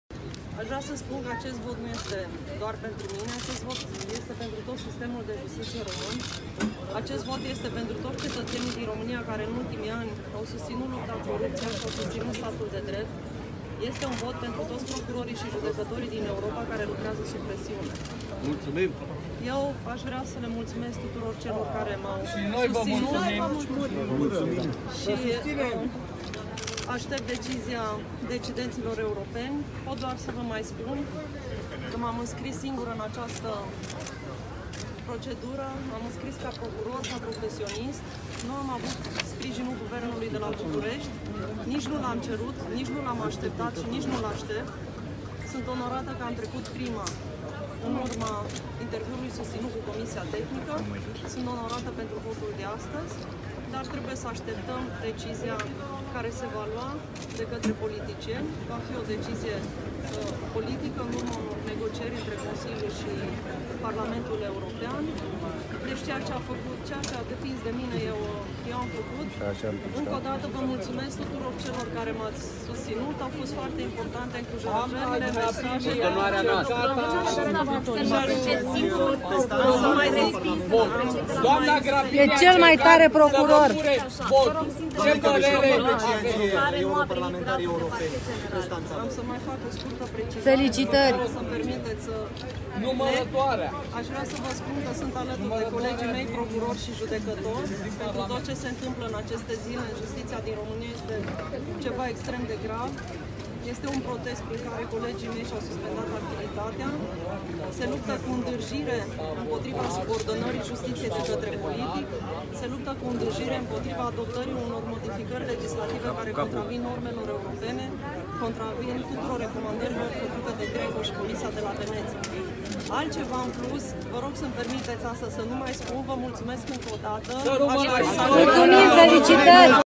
Mediafax -Kovesi,la întoarcerea în ţară:Decizia privind procurorul-şef european va fi politică
Mediafax 27 Februarie: Laura Codruţa Kovesi a declarat miercuri, la revenirea în ţară de la Bruxelles, că decizia privind procurorul şef european va fi una politică, urmare a unor negocieri şi că a mers ca profesionist să candideze, neavând susţinerea Guvernului român.